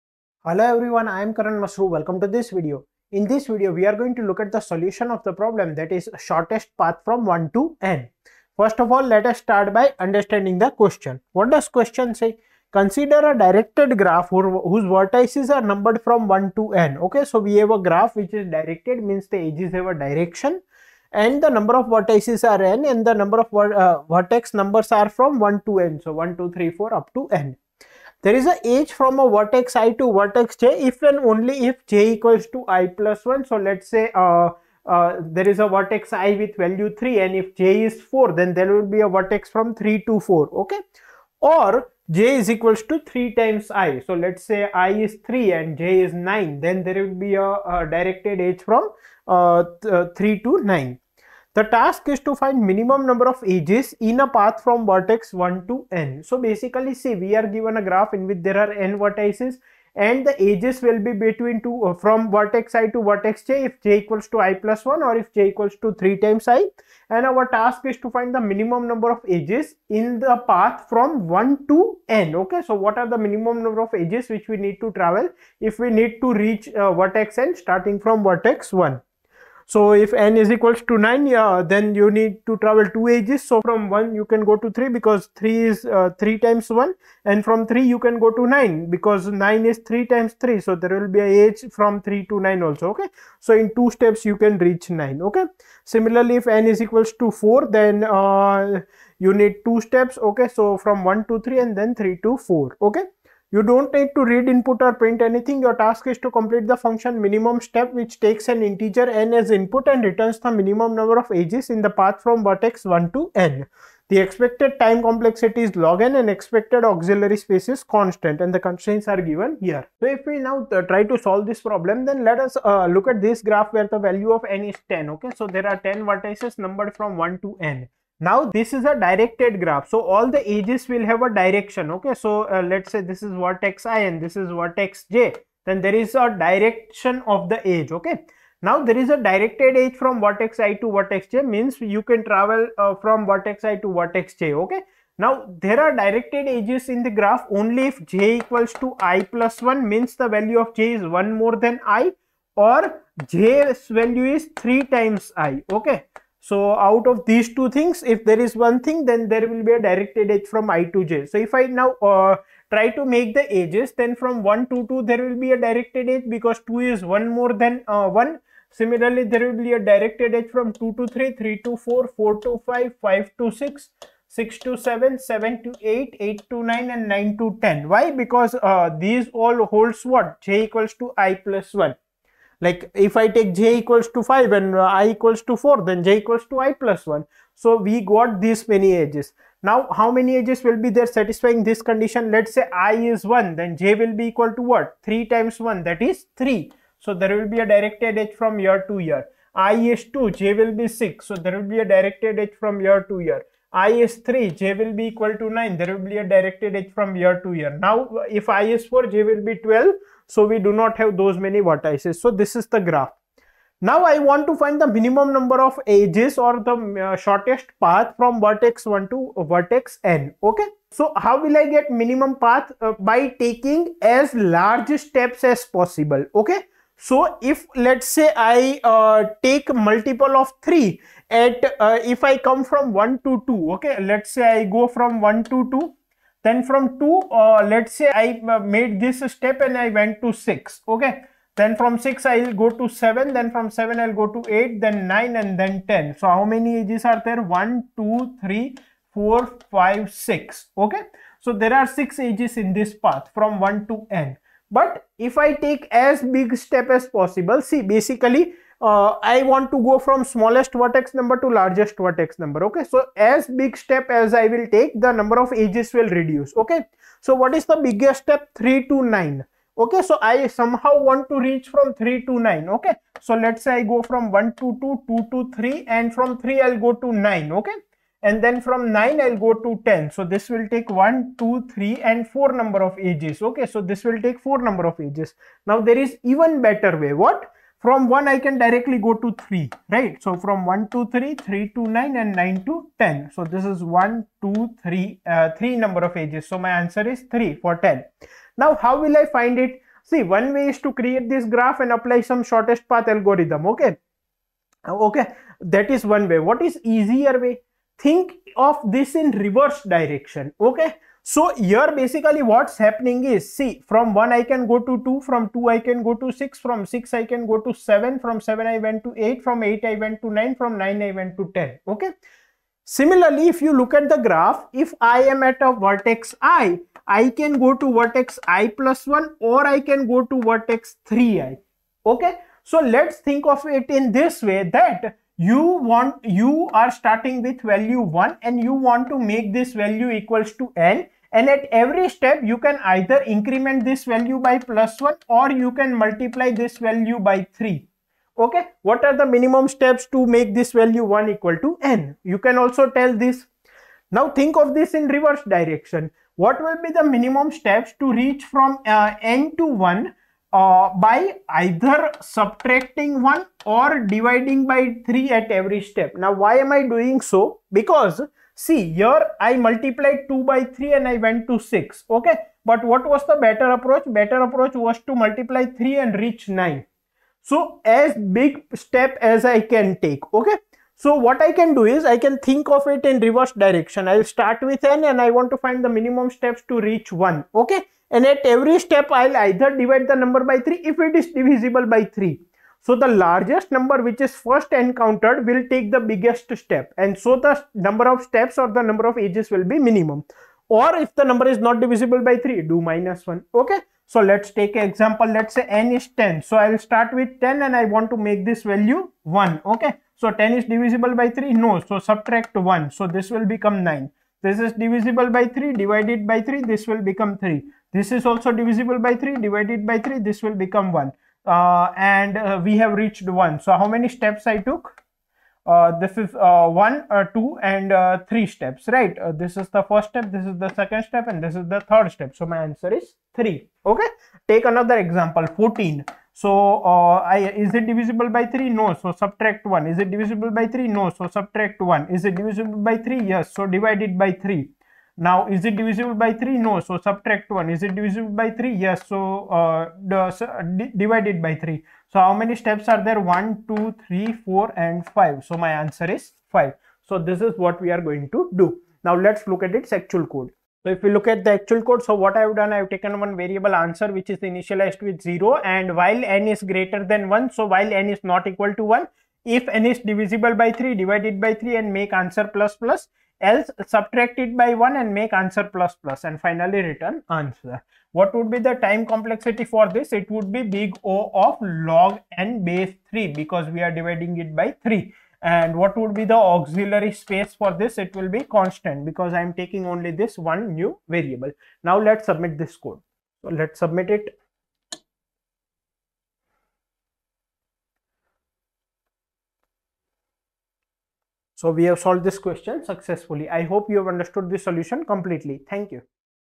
POTD- 30/11/2023 | Shortest path from 1 to n | Problem of the Day | GeeksforGeeks
Welcome to the daily solving of our PROBLEM OF THE DAY with Karan Mashru. We will discuss the entire problem step-by-step and work towards developing an optimized solution. This will not only help you brush up on your concepts of Data Structures and Algorithms but will also help you build up problem-solving skills.

So come along and solve the GFG POTD of 30th November 2023 with us!

Find daily solutions for POTD here on our channel!